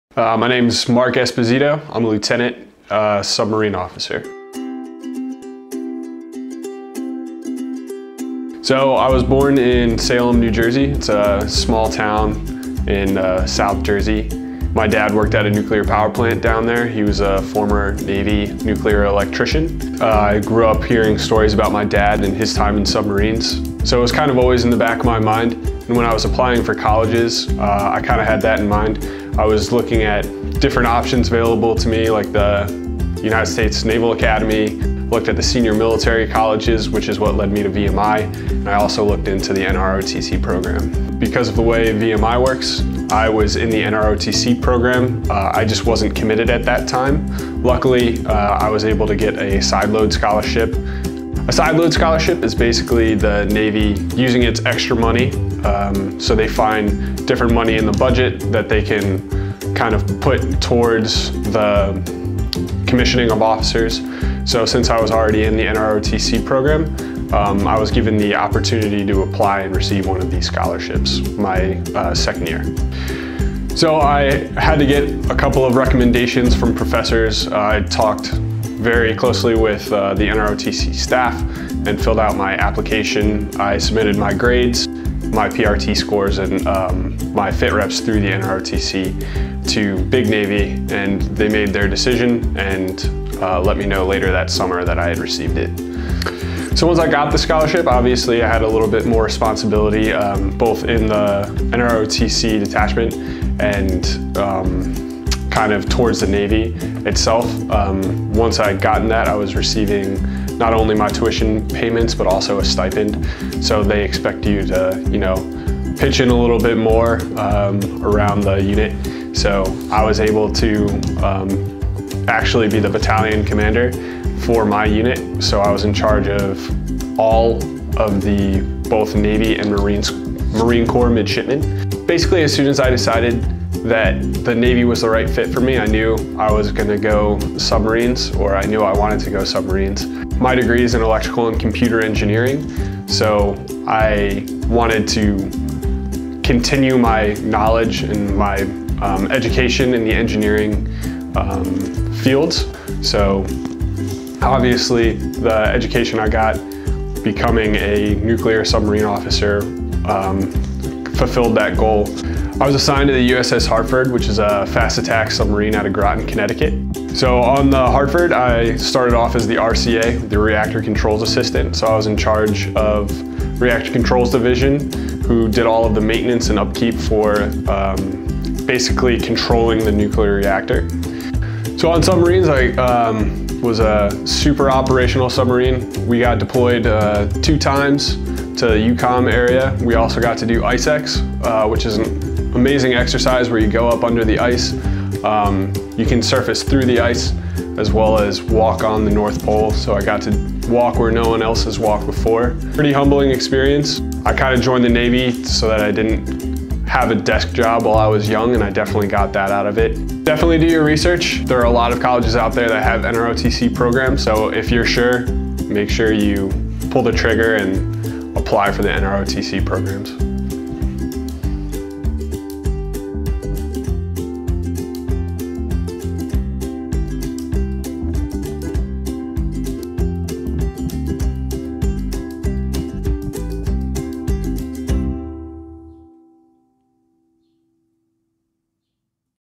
Navy ROTC explanation with LT. Mark Esposito
Learn how Mark Esposito went from the Virginia Military Institute to running the Reactor Controls division onboard a nuclear submarine